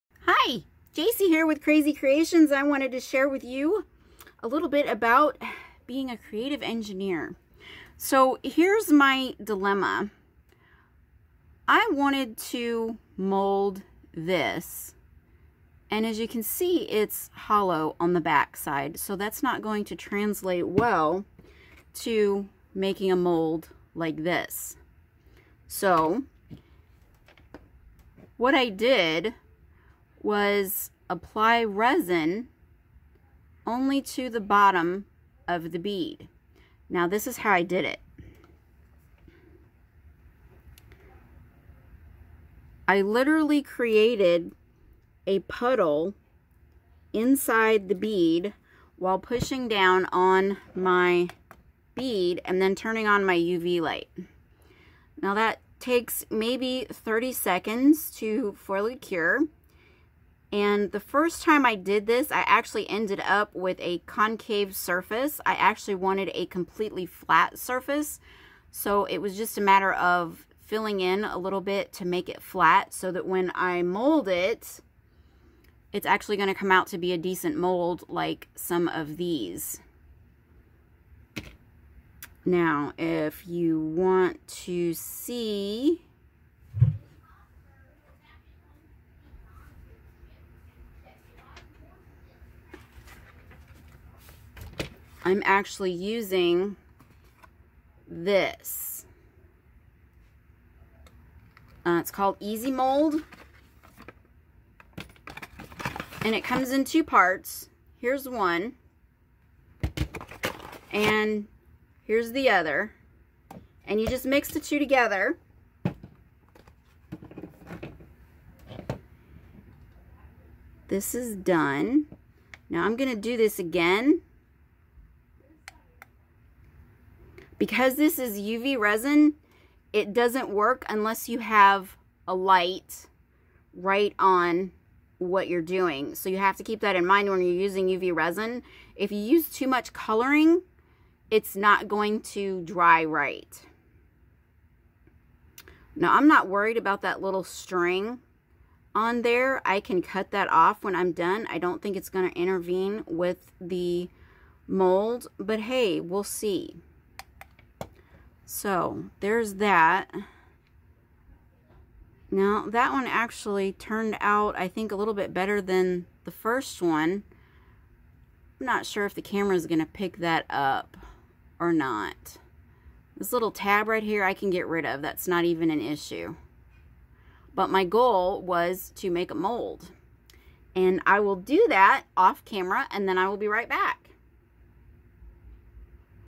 Creative engineering beads to mold
My solution to a hollow bead I needed to mold. Products used are Easy Mold (silicone putty) and BlueMoon UV resin, UV light and silicone mat.
Do not put the silicone mold material on the silicone mat until it's fully cured.